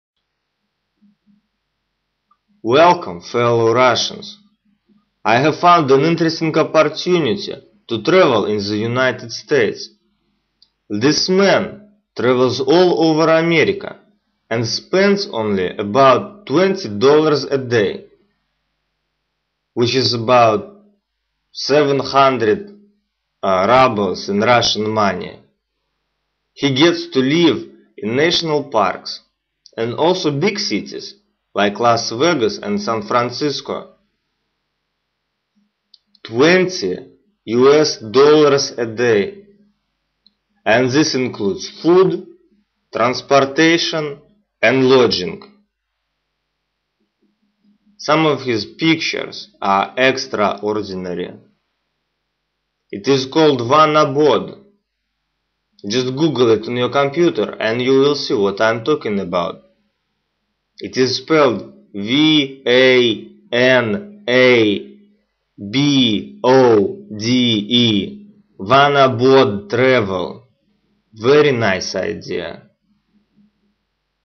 vanabode-russia.wmv
Russian radio personality reviews Vanabode to English speaking people from the Mother Country. Funny stuff!